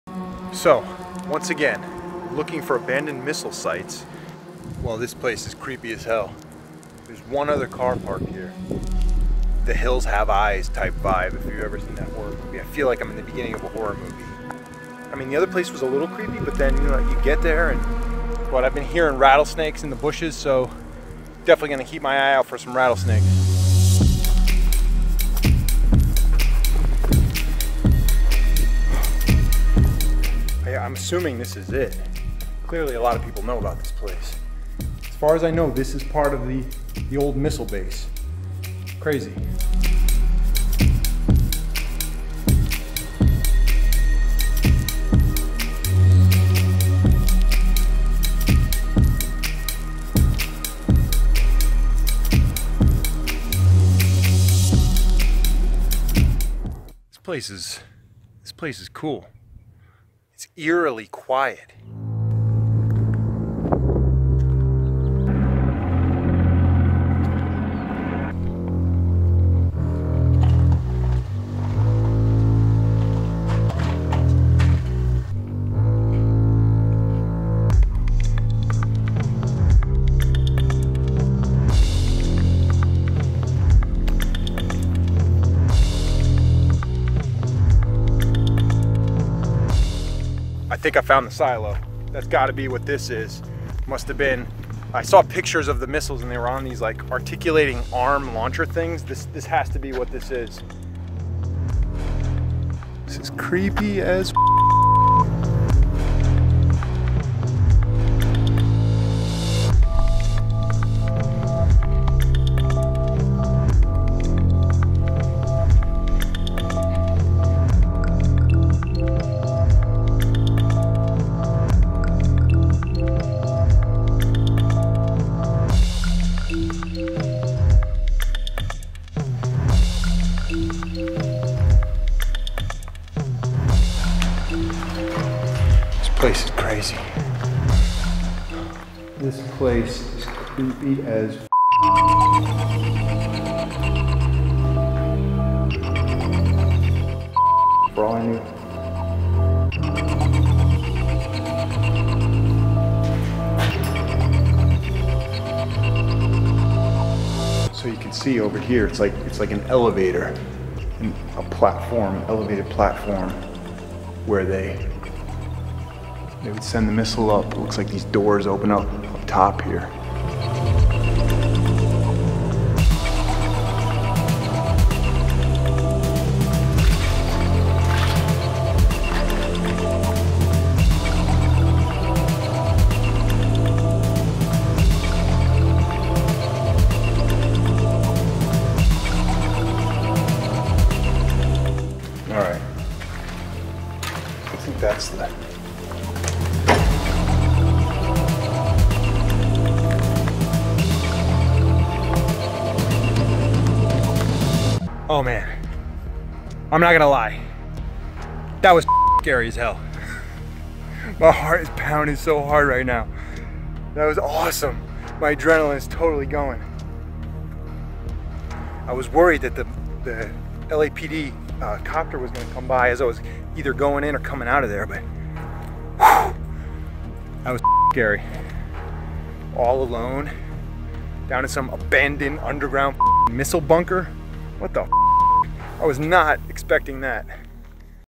Exploring Abandoned Missile Launch Site
ID: 1603820

*MANDATORY ON SCREEN CREDIT - Jesse St Louis*

An urban explorer took a tour around an abandoned missile base left over from the Cold War.

When actor Jesse St Louis set out to visit the LA-88 Nike Missile Launch Site in Chatsworth, California, he wasn’t sure what he’d find.

Walking through the graffiti stained walls, the 41-year-old finally made it to an underground bunker after climbing down a hatch.

Stepping out into the pitch black, Jesse walked around underground for a while where he found broken power lines and graffiti that read ‘leave while you can’.